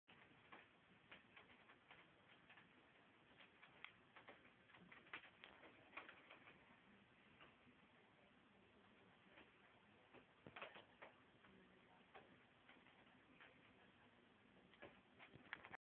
Mari the cat
My new kitty, Mari, being adorable. She's really skinny right now because she was a stray and had kittens, but I'm working hard to fatten her up.